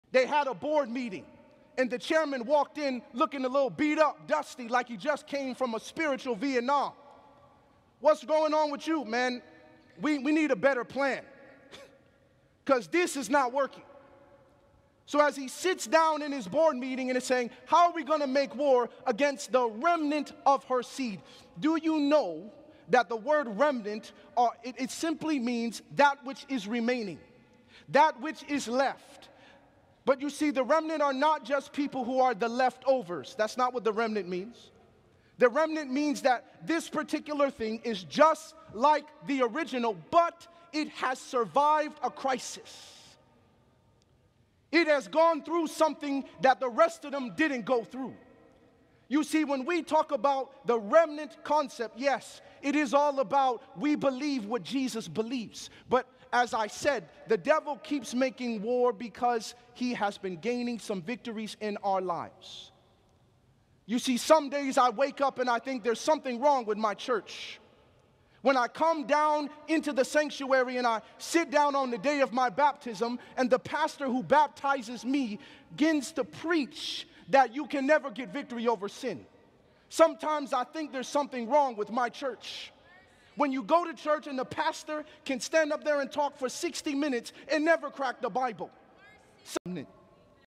Unveiling the Remnant How to Survive and Thrive in a Crisis
This sermon is by Pastor Sebastien Braxton.